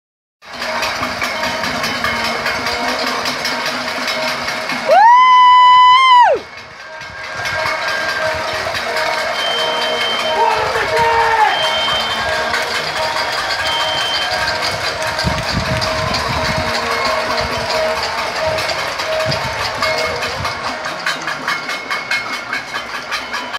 PANELAÇO NA BARRA DA TIJUCA 05/05/2015
Panelaço contra Dilma e PT. Barra da Tijuca - Rio de Janeiro/RJ